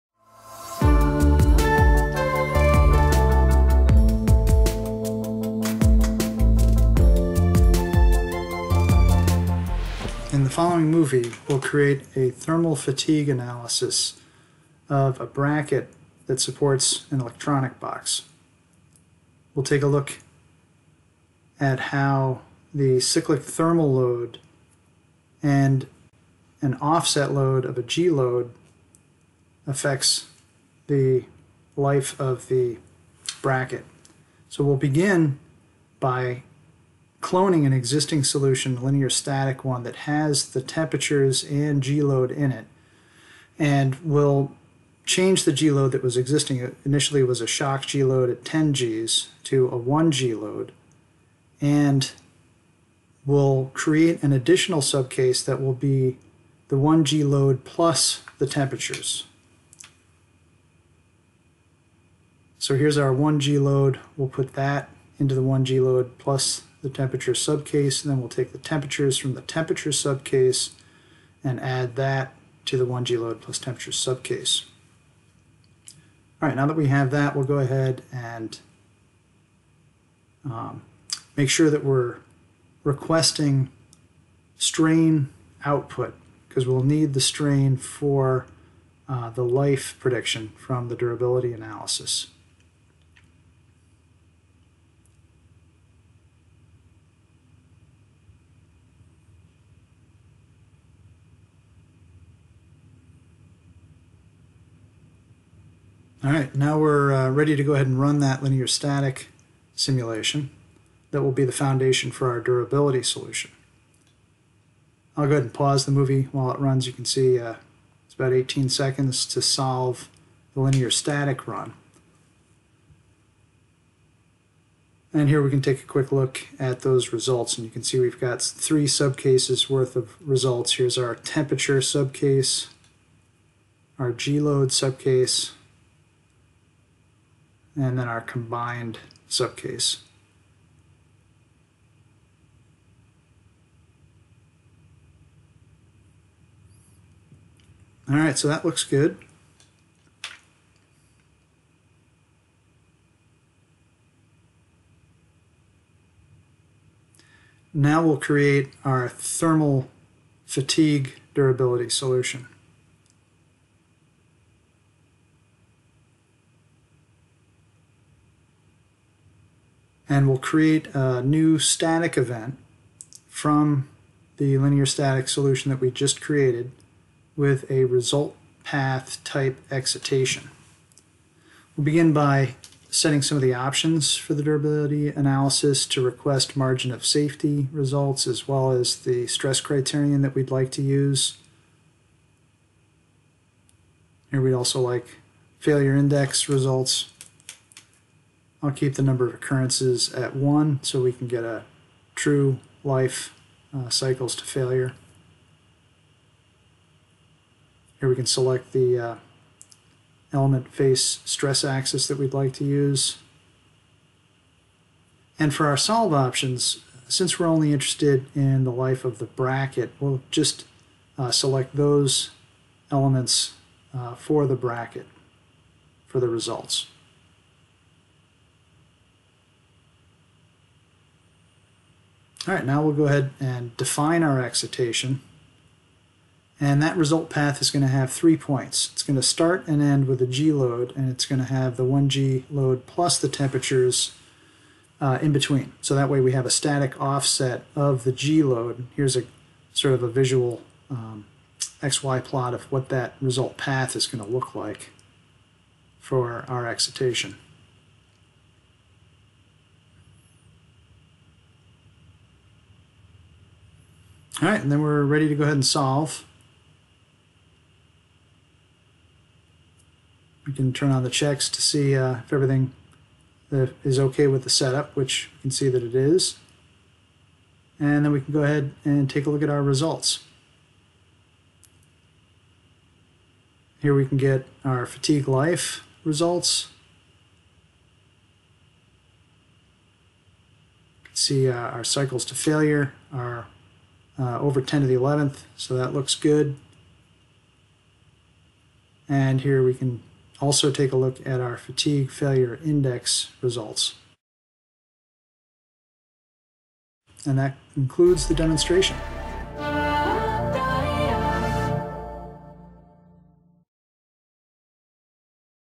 Simcenter 3D Thermal fatigue analysis of a FADEC electronic box
Predict the life of your electronic enclosures with Simcenter 3D Specialist Durability